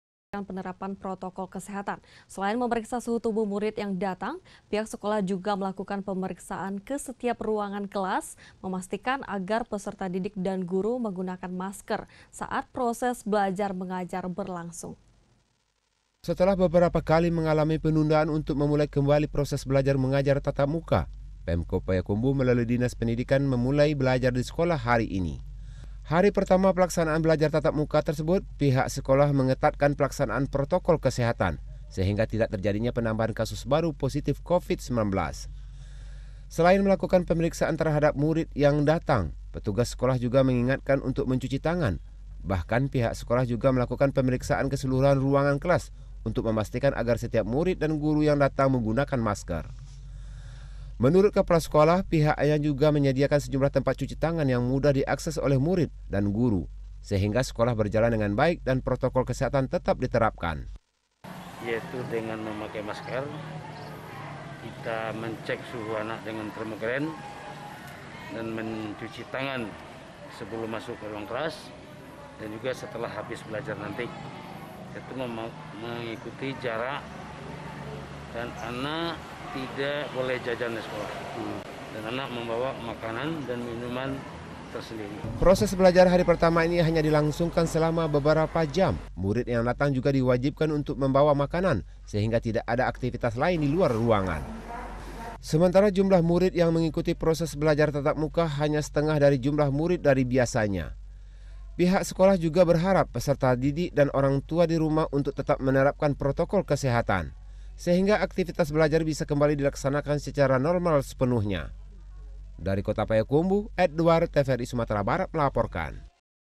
SEKOLAH HARI PERTAMA TATAP MUKA DI PAYAKUMBUH
Pemko Payakumbuh Memulai Sekolah Tatap Muka setelah sebelumnya dilakukan secara daring.

mengantisipasi terus terjadi penambahan kasus positif Covid-19, pihak sekolah mengetatkan penerapan Protokol Kesehatan.